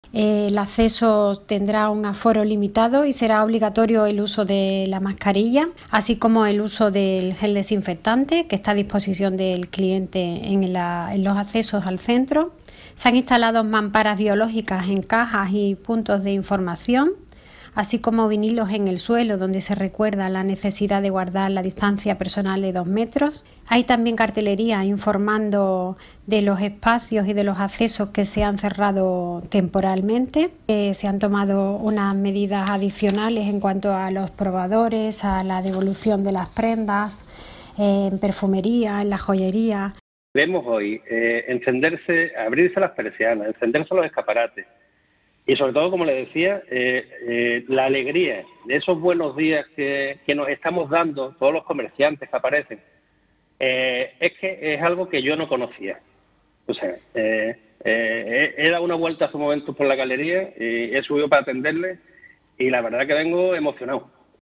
Tras permanecer cerrados durante dos meses los centros comerciales vuelven a abrir sus puertas
El regreso de la actividad, se llevará a cabo extremando las medidas de seguridad e higiénico-sanitarias, para prevenir posibles contagios